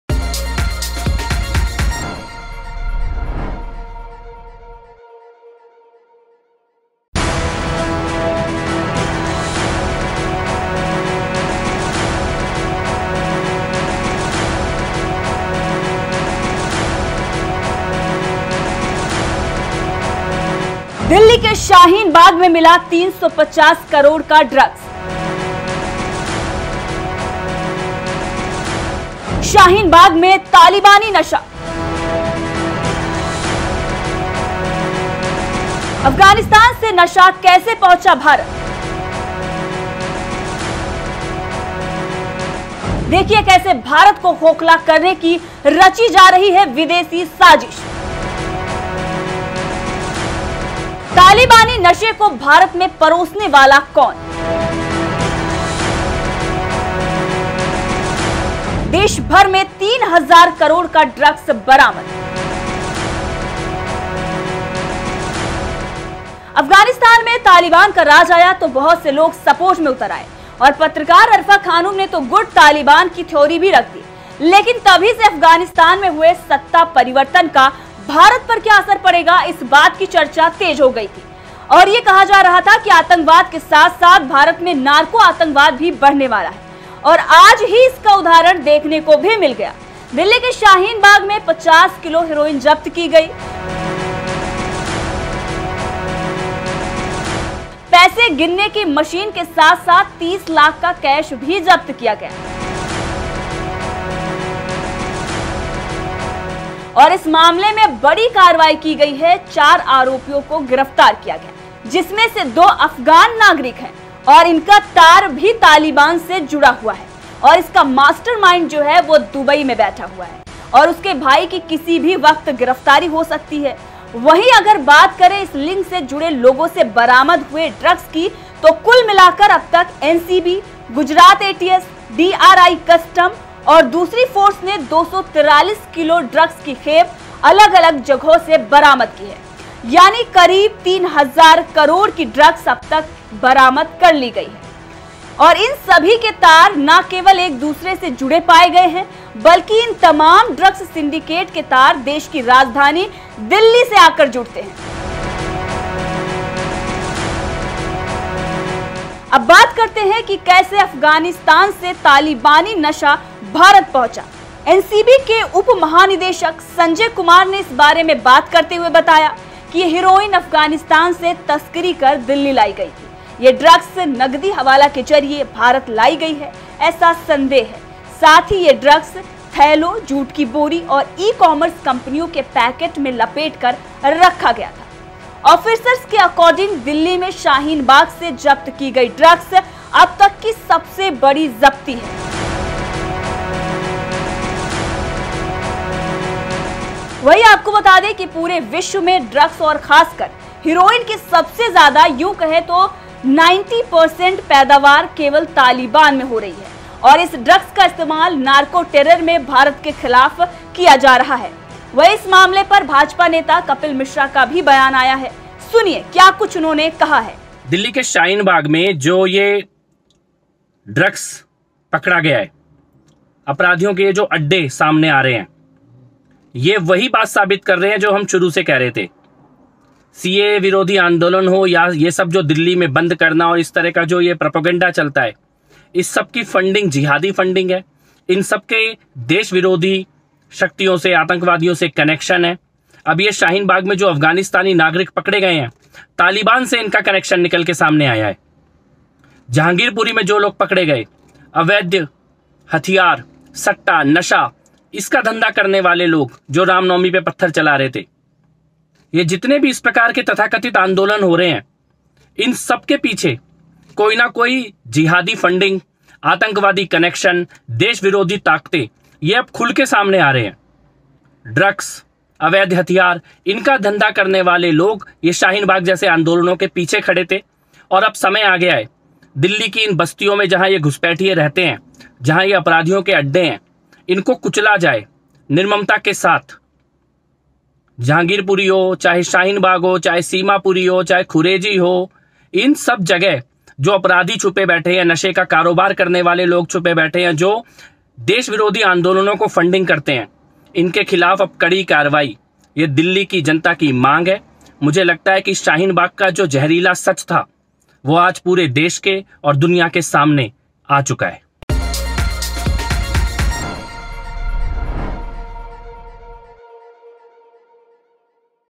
दिल्ली के शहीन बाग़ में आया करोड़ों का ड्रग्स ज़िम्मेदार कौन?
अफ़ग़ानिस्तान में तालिबान का राज आया तो बहुत से लोग सपोर्ट में उतर आए और पत्रकार अरफ़ा खानुम ने तो गुड तालिबान की थ्योरी भी रख दी लेकिन तभी से अफ़ग़ानिस्तान में हुए सत्ता परिवर्तन का भारत पर क्या असर पड़ेगा इस चर्चा तेज़ हो गयी थी। और ये कहा जा रहा था आतंकवाद से साथ साथ भारत में नार्को आतंकवाद भी बढ़ने वाला है और आज ही इसका उदाहरण देखने को भी मिल गया है .

Anchal
Uma